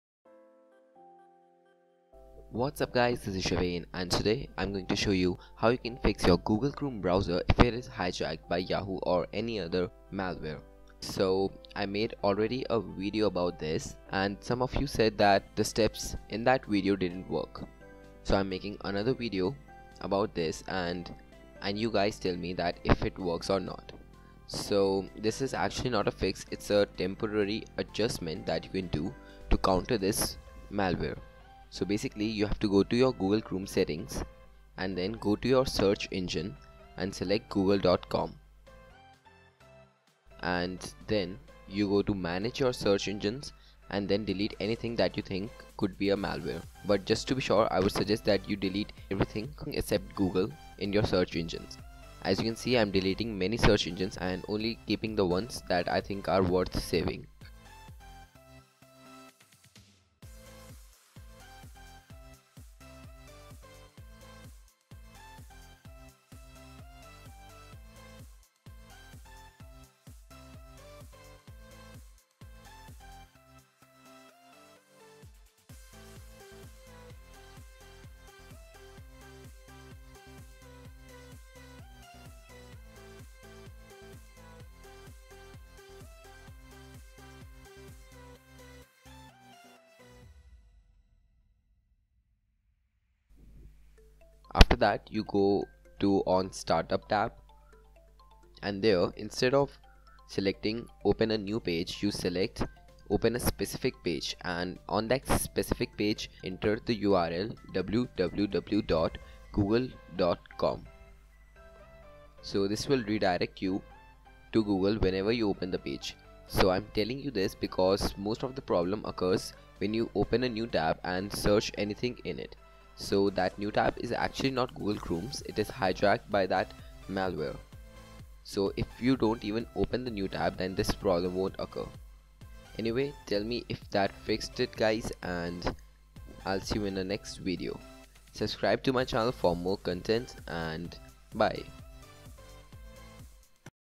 HOW TO FIX Google Chrome Search after Hijack PART 2 - The Struggle Continues...
UPDATE: If you have any computer/software-related problems, please feel free to submit your problem on our new and we will make a solution video for you to resolve it! Hello guys. In this video, we will show you guys how to Google Chrome Search Engine to Google after being hijack by yahoo or some other search engine due to spigot malware which causes this or some other unwanted changes to the web browser. This annoying spigot malware keeps Google Chrome search default to yahoo or any other unwanted search engine.

REMEMBER TO CHECK THE FIRST PART OF THIS!

In this video, we will show you a very simple and easy method.
NOTE: IT IS NOT A PERMANENT FIX, BUT A TEMPORARY FIX.